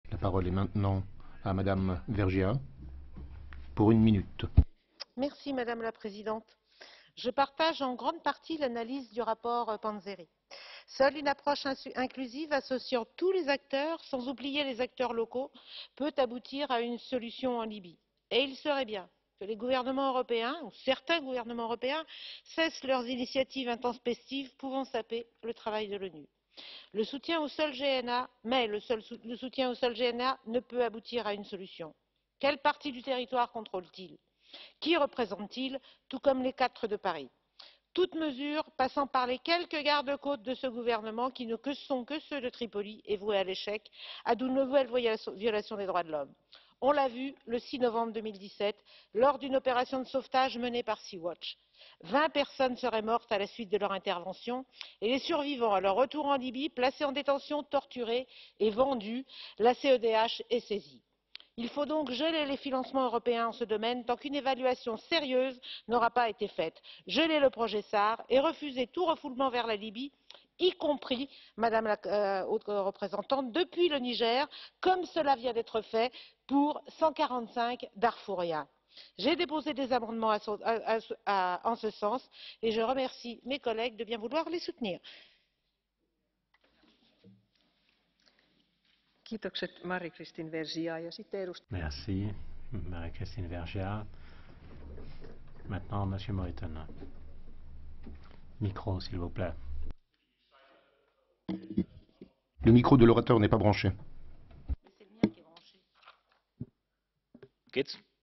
Libye : Il faut refuser tout refoulement vers la Libye et geler les financements européens
Je suis intervenue en session plénière le mardi 29 mai 2018 au sujet de la Libye. J'ai déposé un certain nombre d'amendements dont quatre qui ont été adoptés et inclus au rapport final pour insister notamment sur les 145 Soudanais qui ont récemment été expulsés du Niger vers la Libye.

Retrouvez mon intervention ci-dessous.

"Madame la Présidente, je partage en grande partie l'analyse du rapport Panzeri. Seule une approche inclusive associant tous les acteurs, sans oublier les acteurs locaux, peut aboutir à une solution en Libye. Il serait bien que les gouvernements européens, certains gouvernements européens, cessent leurs initiatives intempestives pouvant saper le travail de l'ONU. Mais le soutien au seul gouvernement d’entente nationale ne peut aboutir à une solution. Quelle partie du territoire contrôle-t-il? Qui représente-t-il, tout comme les quatre principaux acteurs du conflit libyen réunis à Paris?

Toute mesure passant par les quelques gardes-côtes de ce gouvernement, qui ne sont que ceux de Tripoli, est vouée à l'échec et risque de déboucher sur de nouvelles violations des droits de l'homme. On l'a vu le 6 novembre 2017, lors d'une opération de sauvetage menée par Sea Watch. Vingt personnes seraient mortes à la suite de leur intervention et les survivants, à leur retour en Libye, auraient été placés en détention, torturés et vendus. La Cour européenne des droits de l’homme a été saisie de cette affaire.

Il faut donc geler les financements européens dans ce domaine tant qu’aucune évaluation sérieuse n'aura pas été effectuée, geler le projet SAR et refuser tout refoulement vers la Libye, y compris, Madame la haute représentante, depuis le Niger, comme cela vient d'être fait pour 145 Darfouriens. J'ai déposé des amendements en ce sens et je remercie mes collègues de bien vouloir les soutenir."